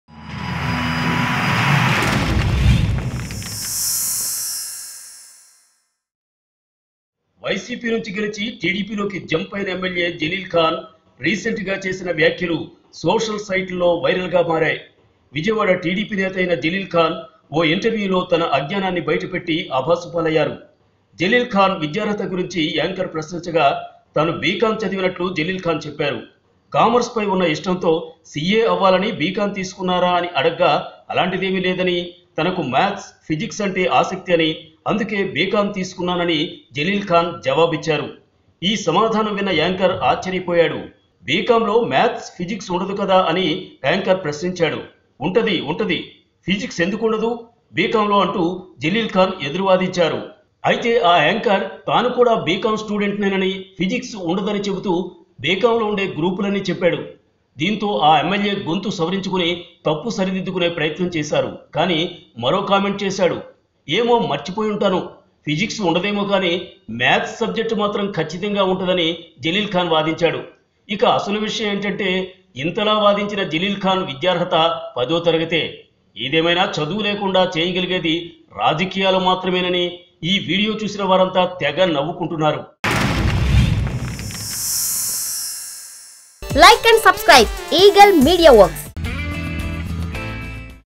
MLA Jaleel Khan Comments ABout His Studies |Jaleel Khan On Bcom | Viral | Eagle Media Works
Watch Jaleel Khan mla Comments ABout Study Jaleel Khan Viral On Social Media  Eagle Media Works.